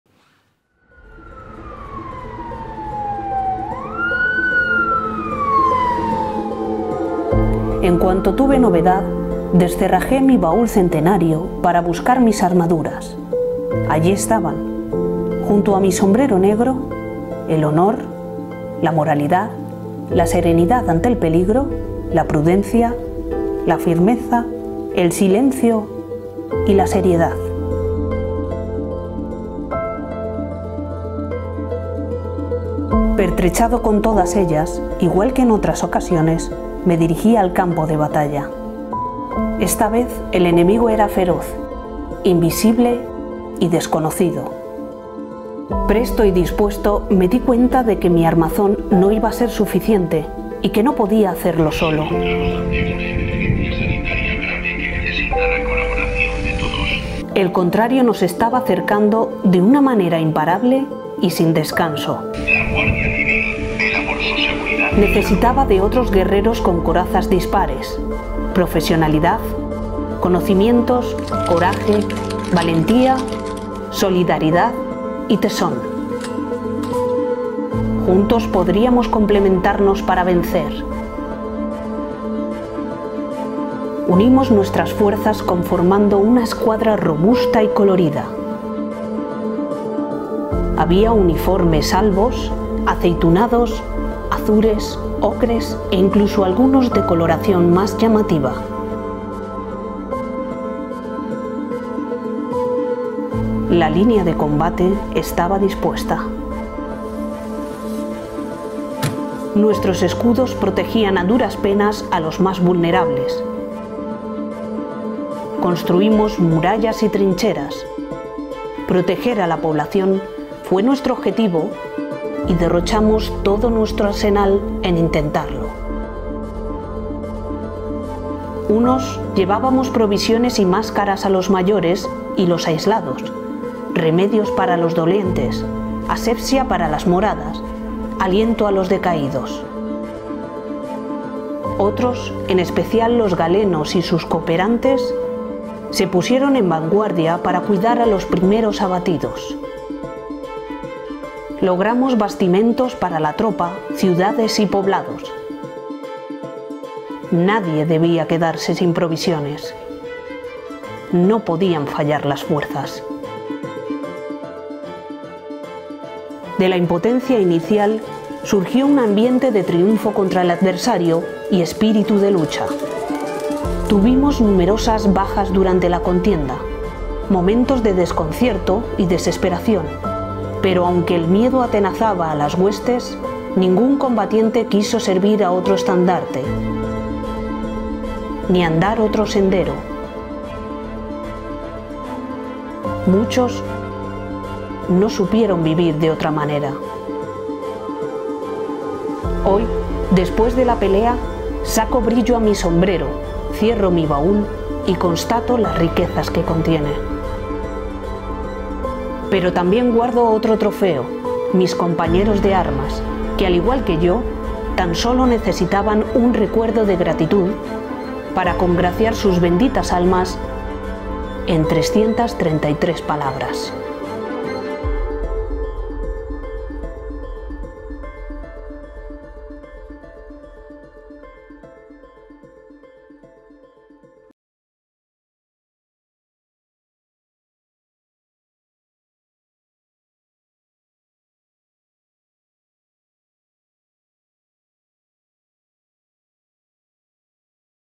Mis compañeros de armas. Premio relato corto Revista Guardia Civil 2021
El fallo del jurado fue sobre la obra de D. Luis Reviriego Chapinal.